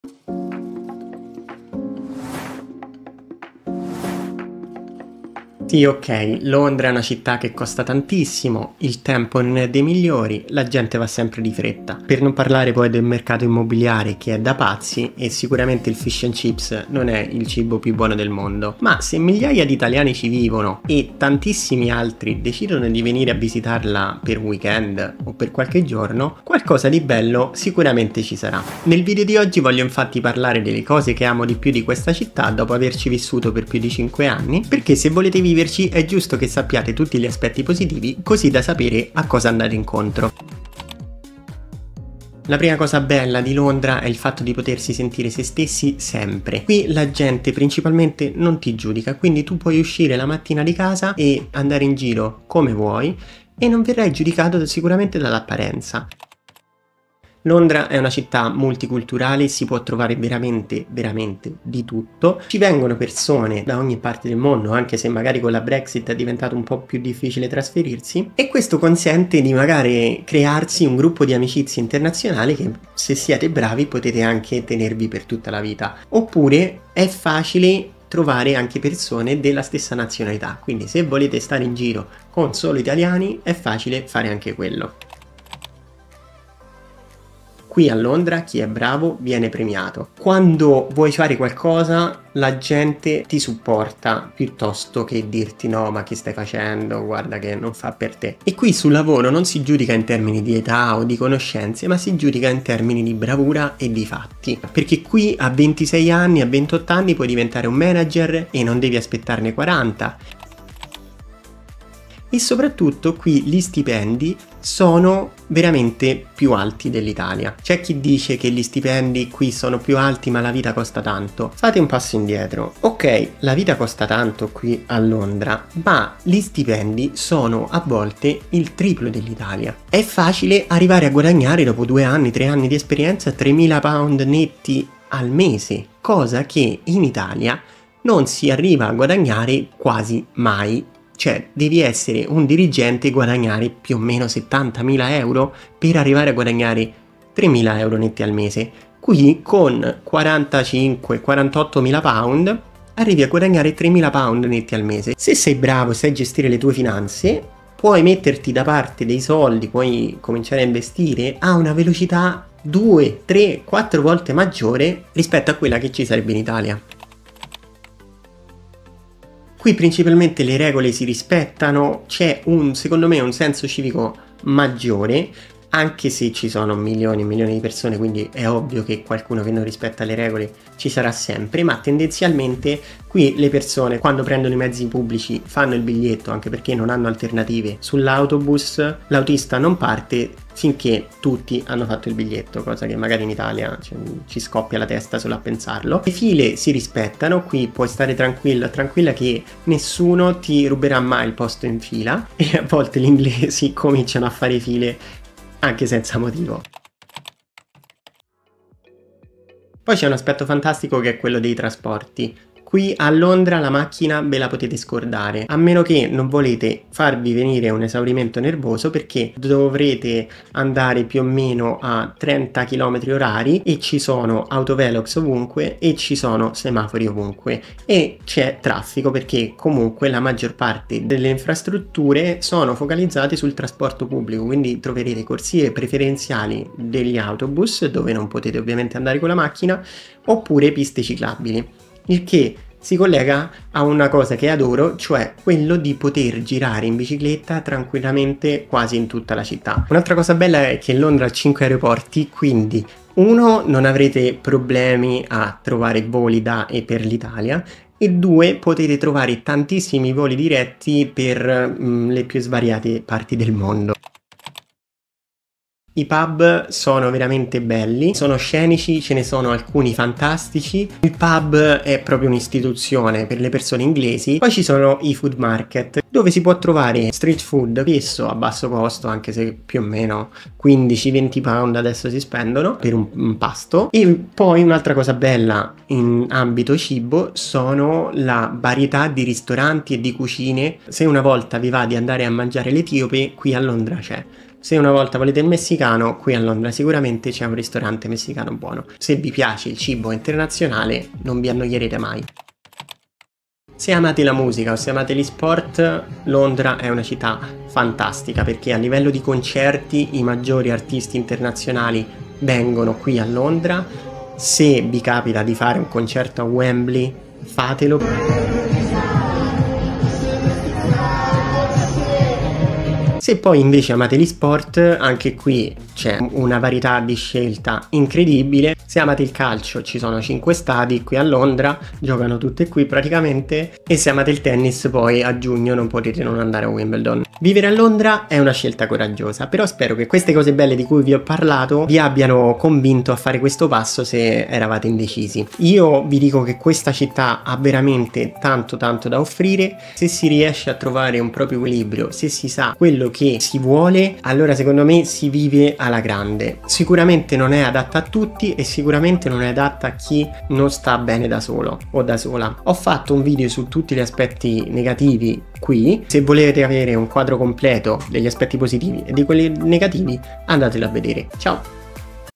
Vivere a Londra - le cose che AMO (guarda prima di trasferirti)
Dopo il video sugli aspetti negativi, oggi vi parlo delle MIE cose preferite di questa città dopo averci vissuto per 5 anni. Quali sono le vostre?

Capitoli:

0.41 Giudizio altrui
0.56 Multiculturalità
1.25 meritocrazia
1.51 stipendi
2.48 rispetto delle regole
3.28 trasporti
4.23 pub & ristoranti
5.02 eventi
5.38 considerazioni

Fatemi sapere nei commenti se secondo voi vale la pena vivere a Londra. 👋